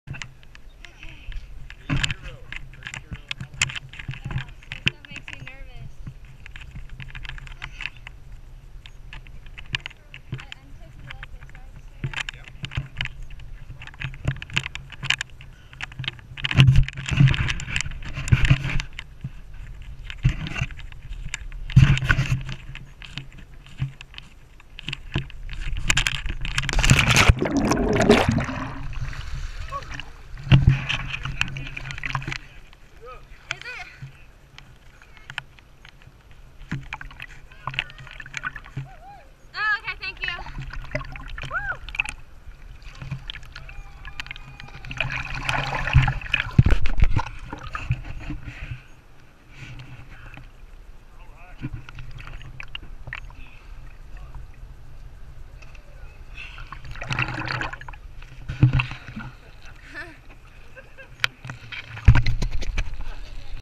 Cliff Jumping at Marble Creek Camp on the Middle Fork of the Salmon River - 07/03/14
Here's Brooke jumping from a cliff at the awesome Marble Creek Camp on the Middle Fork of the Salmon River in Idaho...  This GOPRO video camera, just like this rafting trip on the Middle Fork was definitely some of the best money I have ever spent in my life!